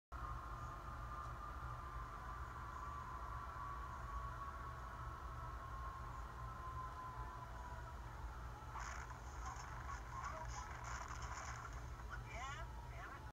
Prius-Kia-8-10-2017-8:40am
Prius V hit by Highlander August 10 8:40am